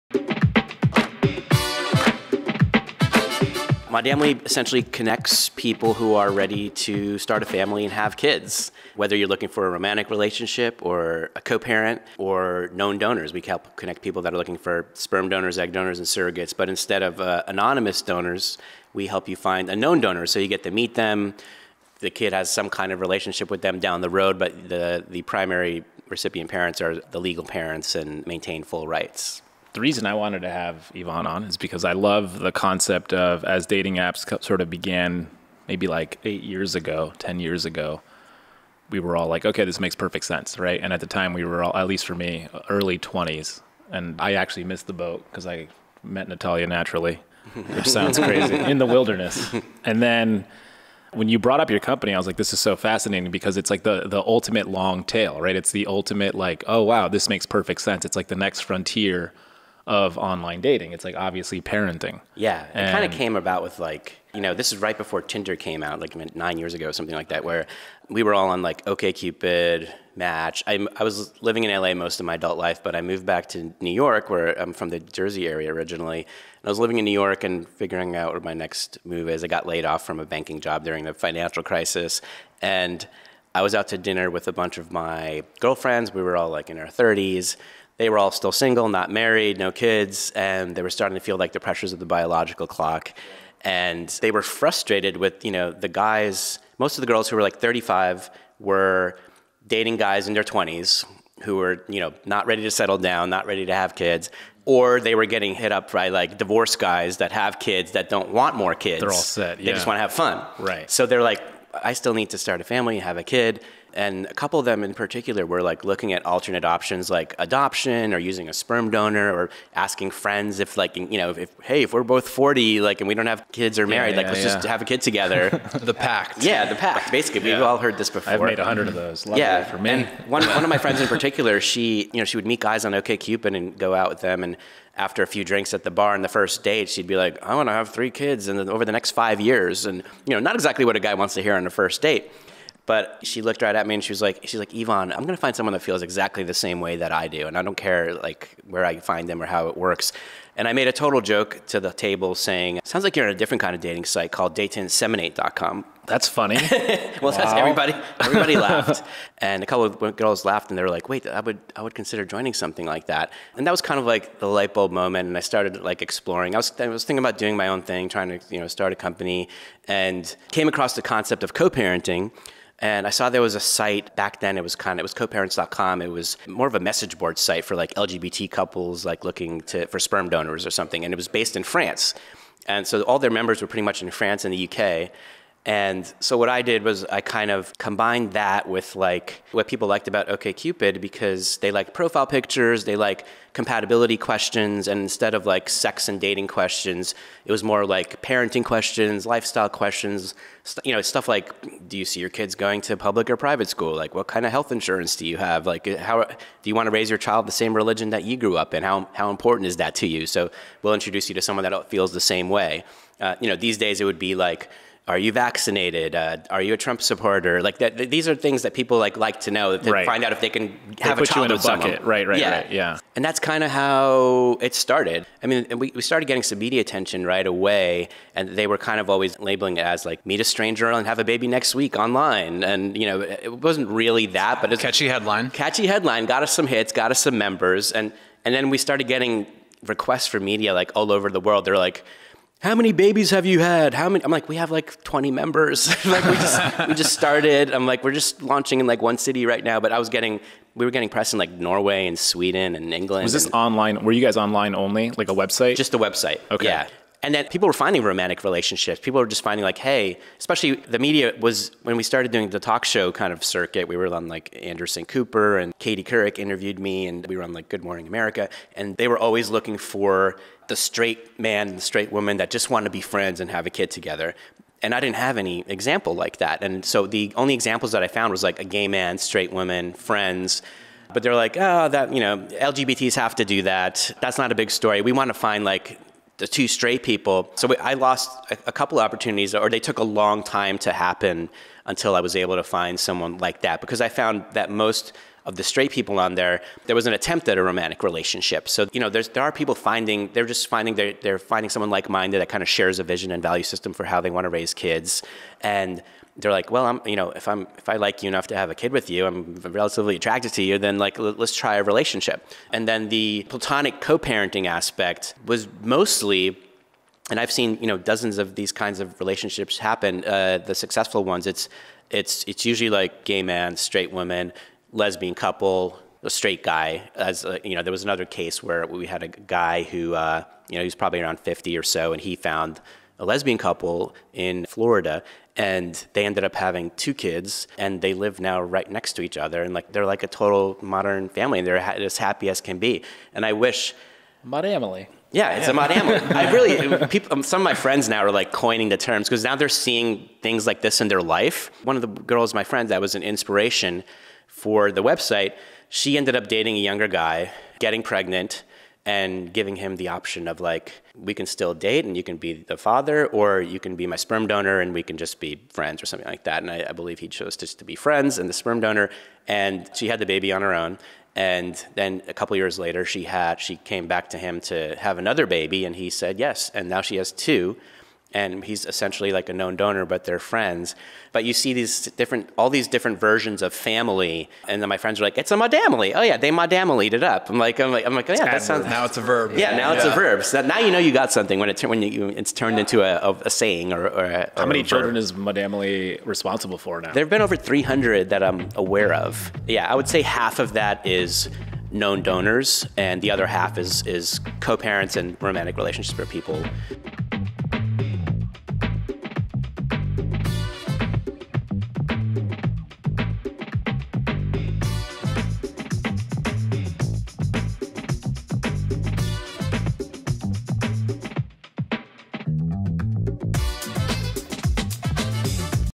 What is Modamily? Answered by the Founder, Ivan Fatovic
Millennial women are having children later in life.  According to Forbes on 5/1/21, this is due to people focusing more on their careers instead of building a family.  However, this poses a serious problem years later.  Once these adults are ready to have children, they may not be already dating someone.

This is what Modamily looks to solve.  Modamily that a service that connects like-minded individuals whose main focus is on having children, or starting a family.  It's Bumble for people ready to have kids.

In this clip of our podcast, Ivan Fatovic, founder of Modamily, explains the app and the impacts it's had on people's lives.  Hint: The implications aren't what you're thinking